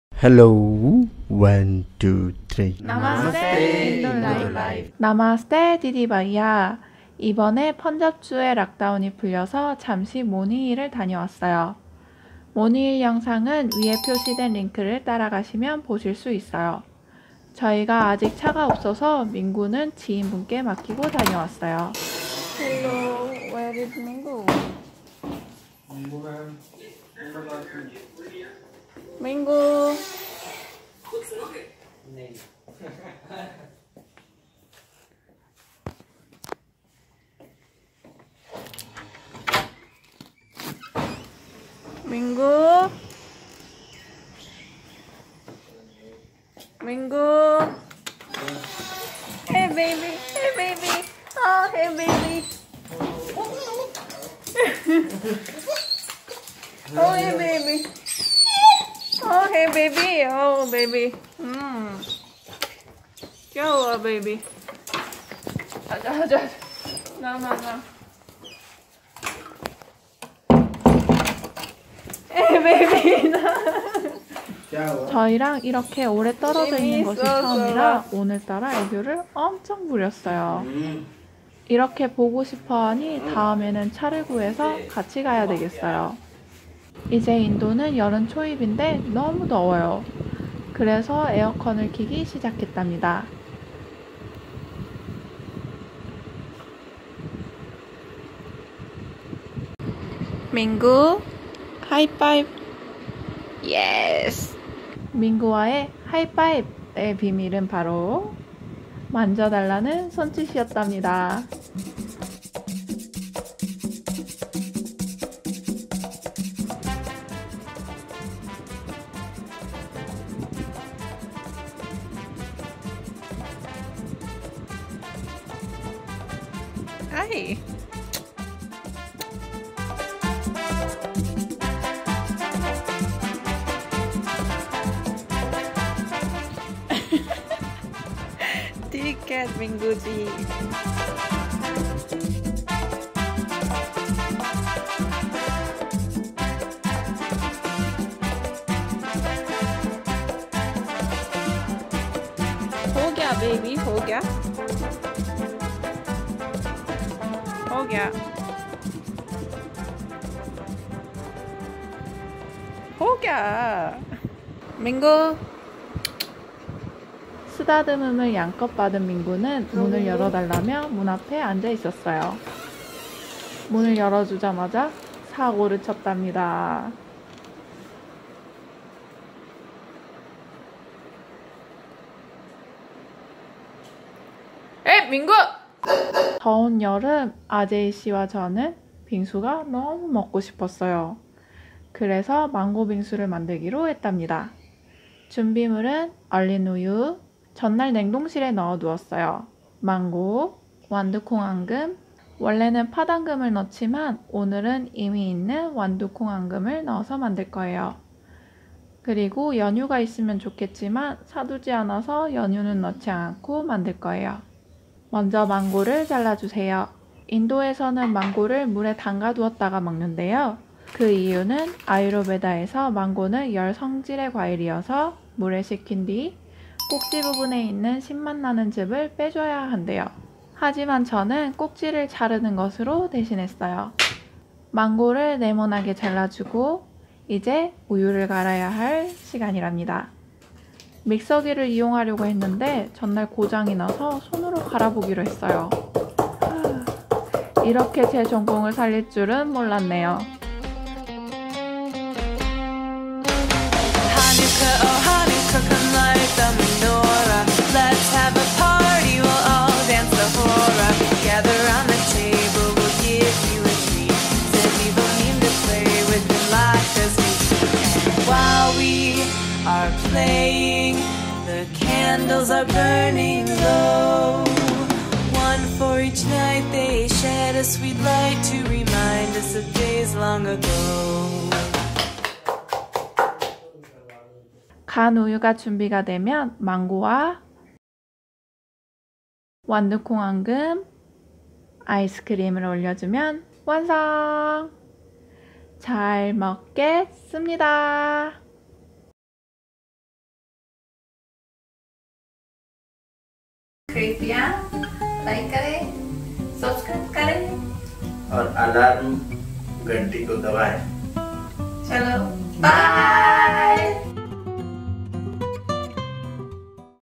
[VLOG] 밍구 일기 & 망고 빙수 만들기
Namaste, Didi Bhaiya

이번에 펀잡 주의 락다운이 풀려서 잠시 모니힐을 다녀왔답니다.
밍구는 지인에게 맡겼는데
처음으로 떨어져 있는거라 그런지 저희가 집에 오자마자 난리가 났답니다.

그리고 더운 날씨에 빙수가 너무나도 먹고 싶었어요.
그래서 망고 빙수를 만들기로 했는데 믹서기가 고장나 버렸답니다.

밍구의 격한 반김과 어떻게 빙수를 만들었는지는 영상에서 확인해주세요~!